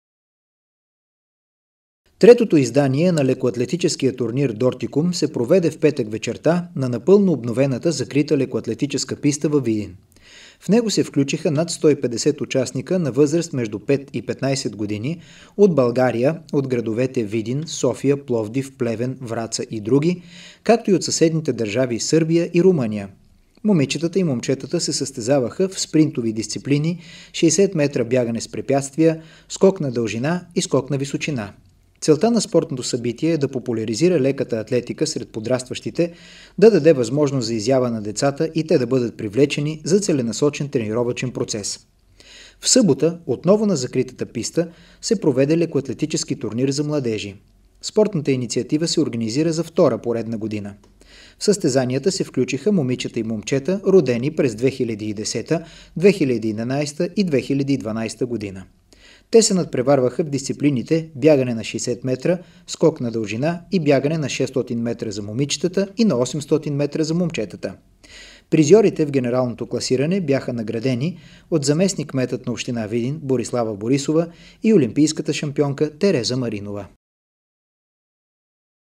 Лекоатлетически уикенд във Видин
Видин бе домакин на традиционния турнир „Дортикум“ и лекоатлетически турнир за младежи. Събитията бяха организирани от Спортен клуб „Радуканови”, община Видин и Българската федерация по лека атлетика.